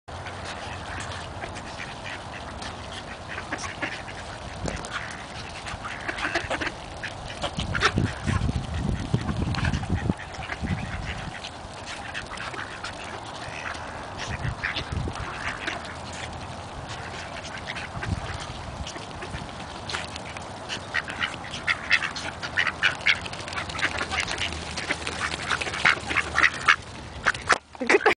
Duck Bites Camera
Take care when you approach the feeding frenzy, lest ye become part of the feast itself!

Mischa and Mosel (the two conspicuously hungry Buff Orpington drakes) are no longer with us, I'm sad to say, but their legendary appetites live on!

(No cameras were harmed in the making of this video.)